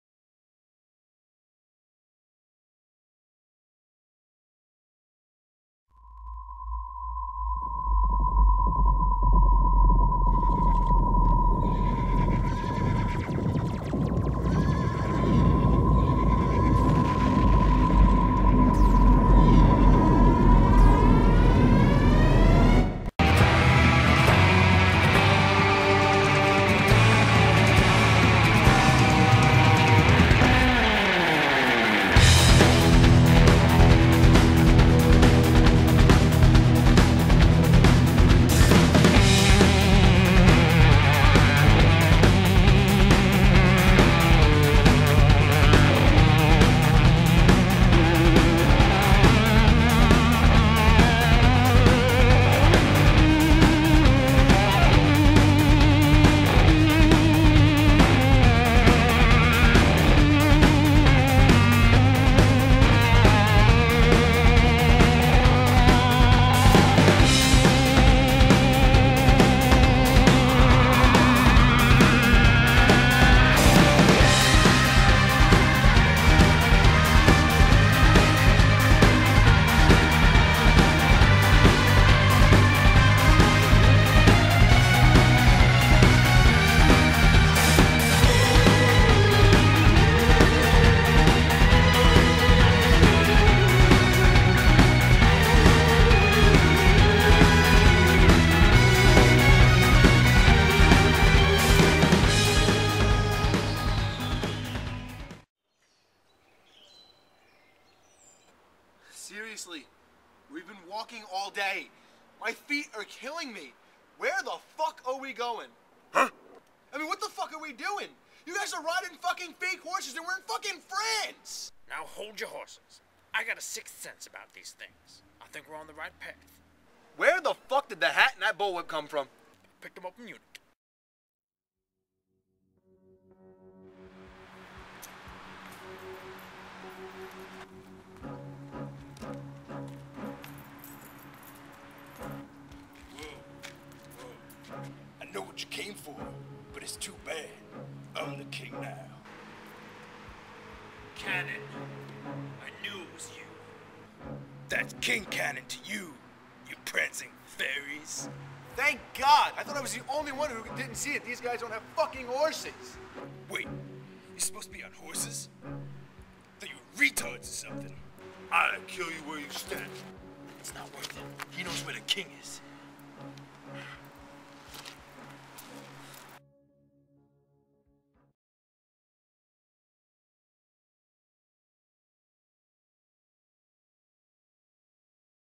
Film No. 3: "LRIGWOC (Pt. 2)"
Part two of the exciting adventure of three men on the quest to find their King!